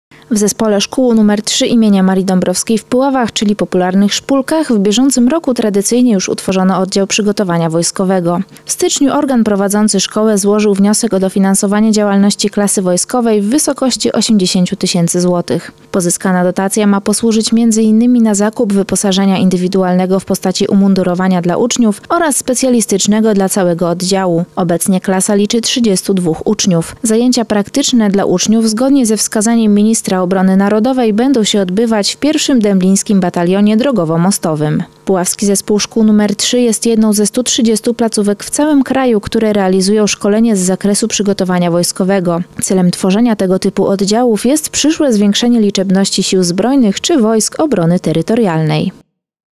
12 02 2021 Dotacja na Oddział Przygotowania Wojskowego w ZS3
W styczniu organ prowadzący szkołę złożył wniosek o dofinansowanie działalności klasy wojskowej w wysokości 80 tysięcy złotych.